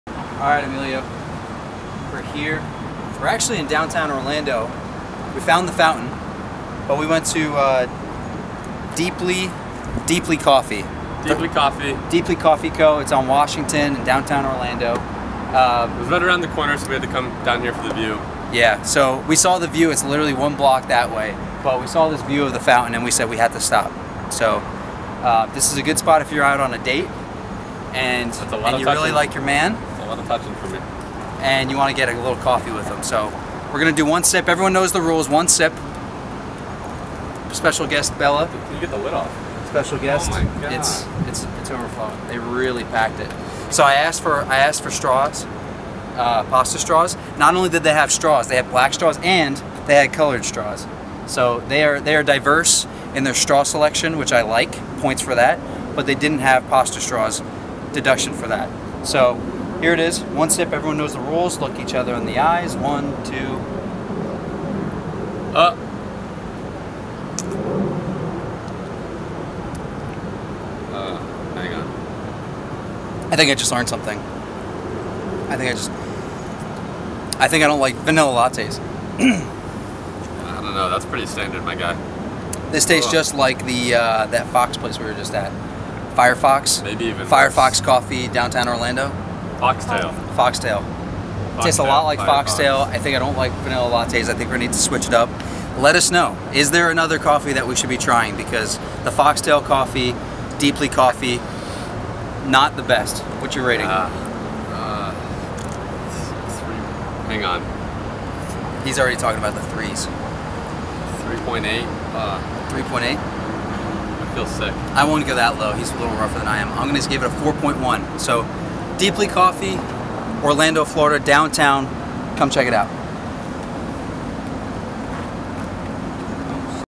Coffee Review #3 Deeply Coffee Co
Coffee Review #3  Deeply Coffee Co. Hunter and Jordan Give their "expert" coffee review on Deeply Coffee